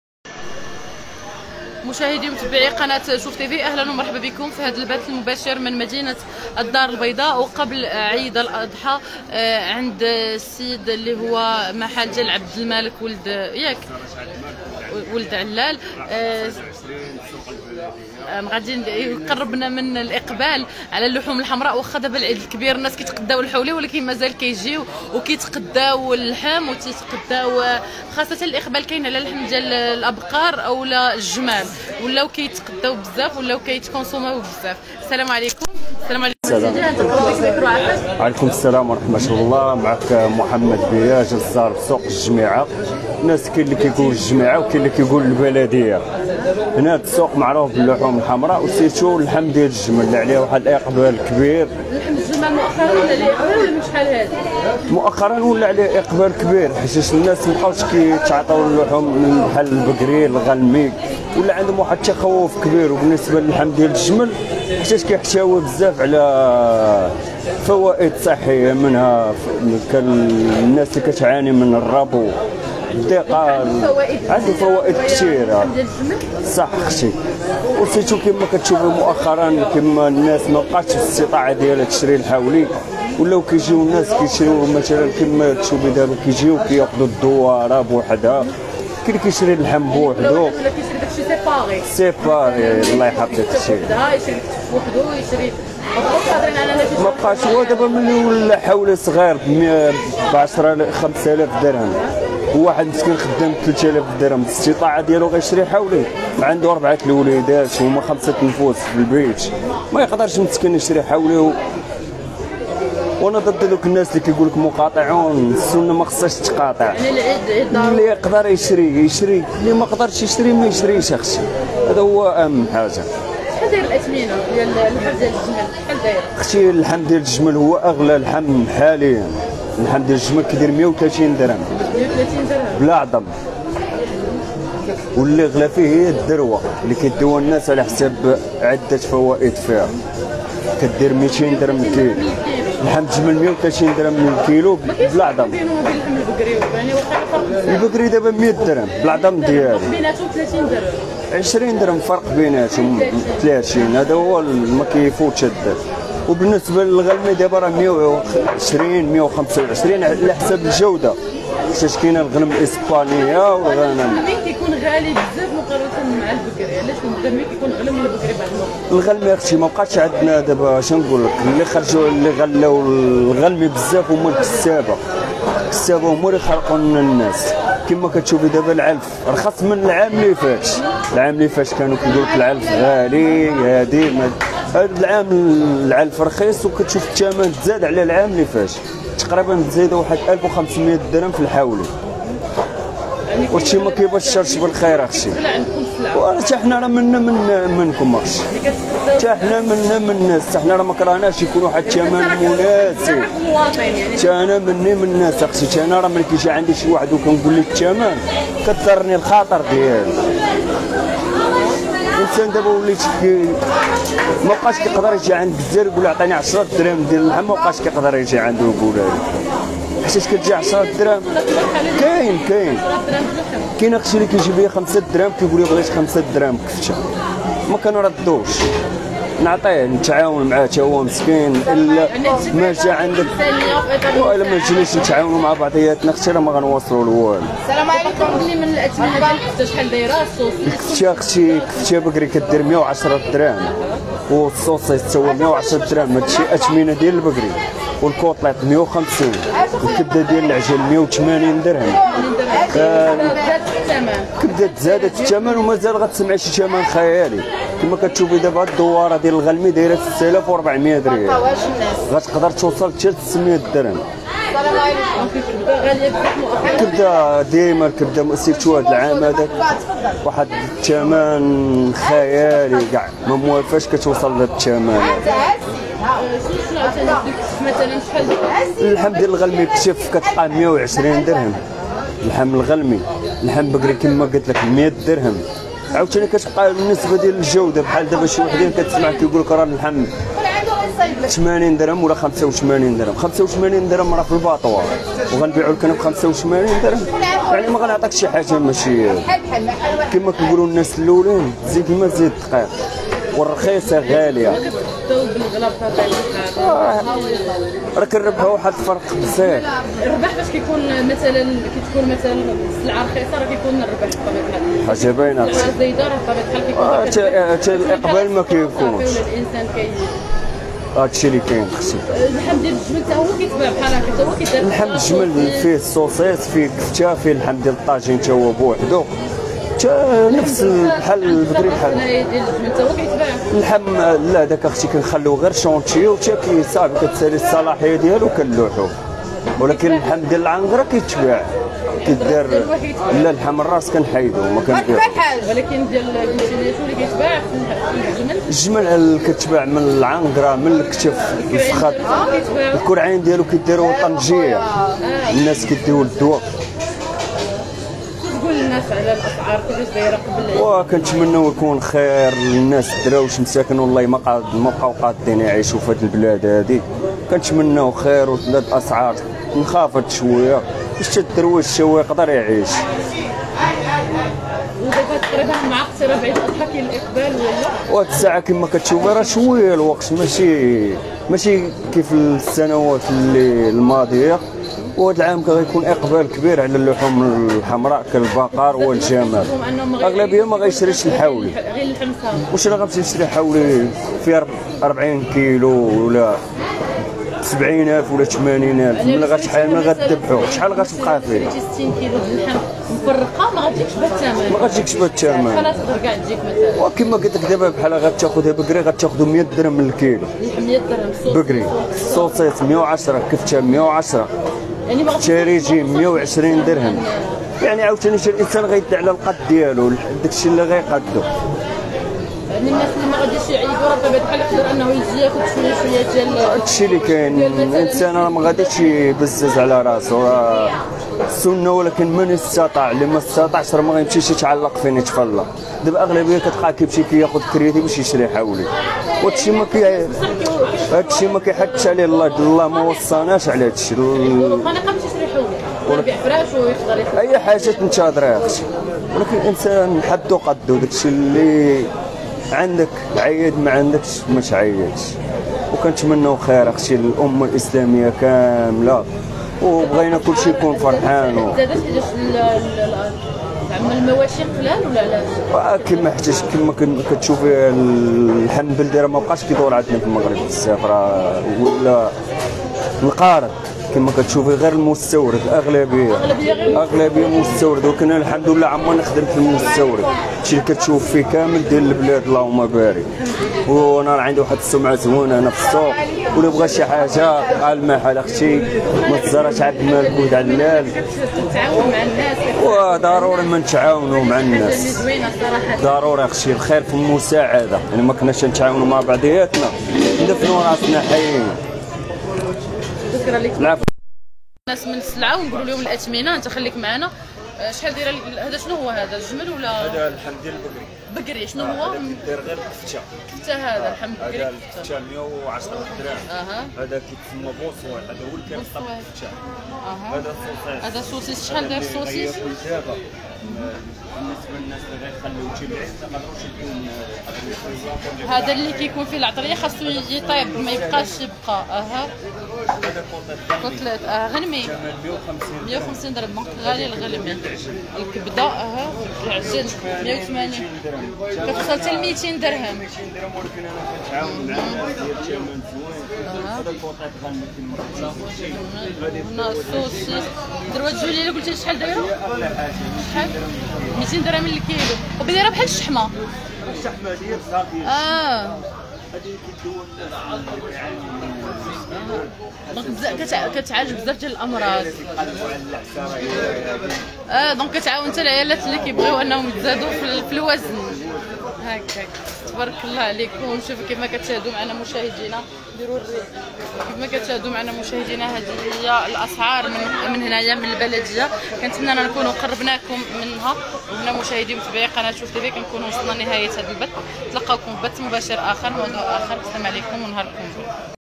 قبل عيد الأضحى: إقبال كبير على اللحوم الحمراء وخاصة لحوم الأبقار والجمال..شوفو شحال وصلات الأثمنة
مباشرة من الدار البيضاء..قبل عيد الأضحى: إقبال كبير على اللحوم الحمراء وخاصة لحوم الأبقار والجمال..شوفو شحال وصلات الأثمنة